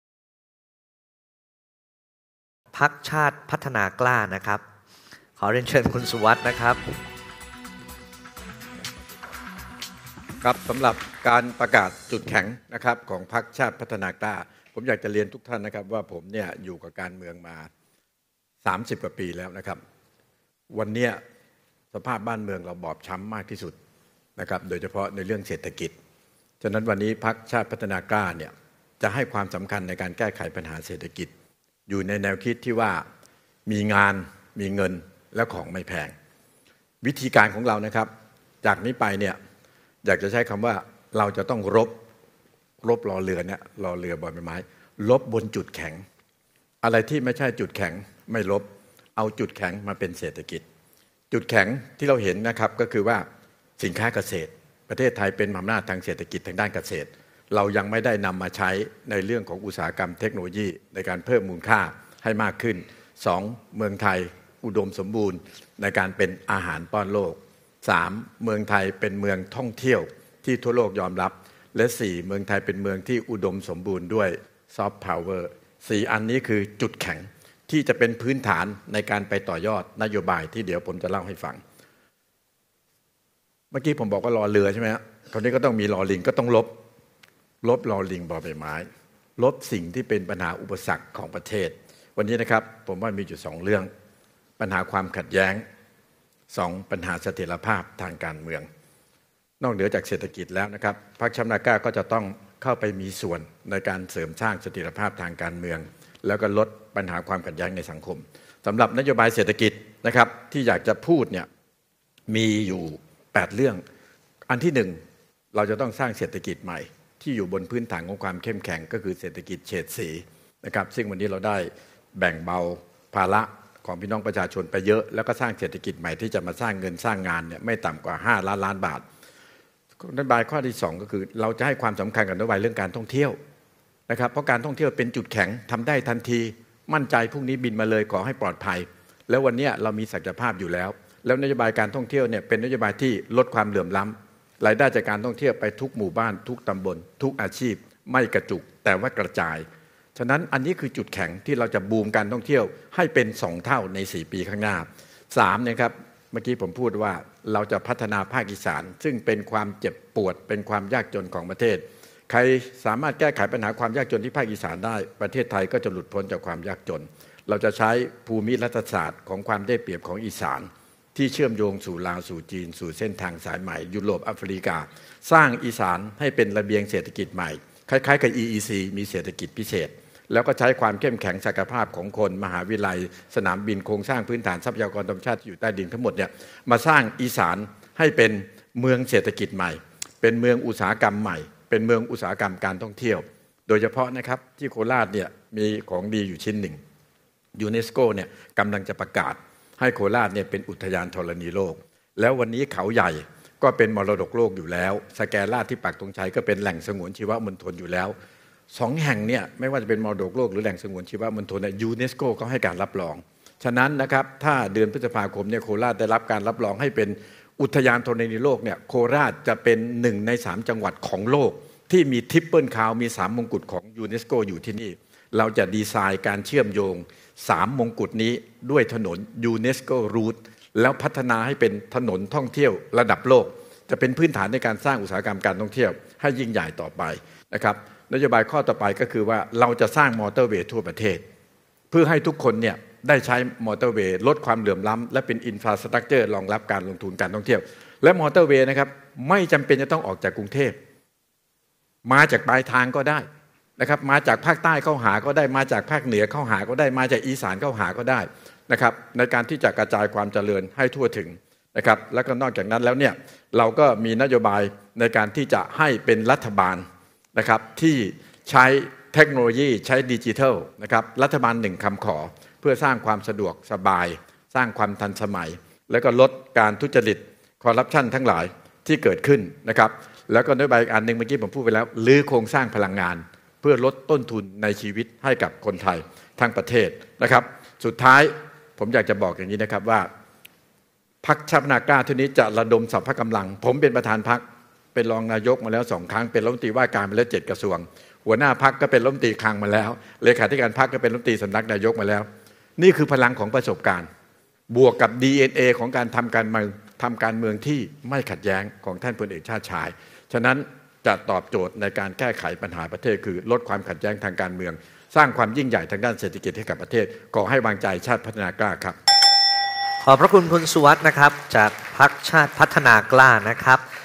"ชูจุดแข็ง รบ และ ลบ สร้างความยิ่งใหญ่ด้านเศรษฐกิจ" : Matichon Online
"ชูจุดแข็ง รบ และ ลบ สร้างความยิ่งใหญ่ด้านเศรษฐกิจ"
“สุวัจน์ ลิปตพัลลภ” ประธานพรรคชาติพัฒนากล้า (ชพก.)  : เวทีแรกของแคมเปญ “มติชน: เลือกตั้ง 2566 บทใหม่ประเทศไทย” วันที่ 13 มีนาคม 2566